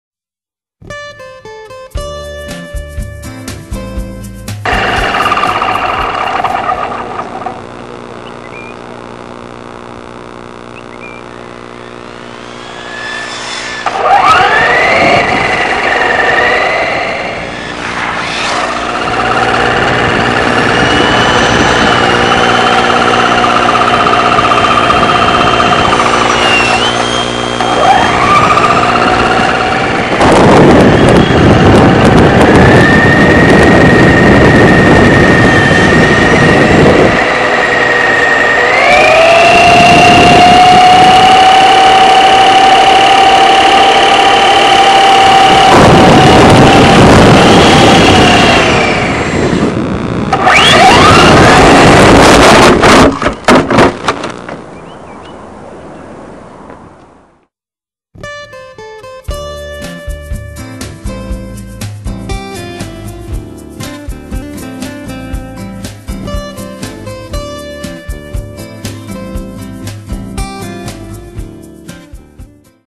Chasing the Red Cat Racing Lightning EPX and the Hobbyking Hoverjet EDF
Park was too wet so we got the flat surface vehicles out and did some parking lot racing..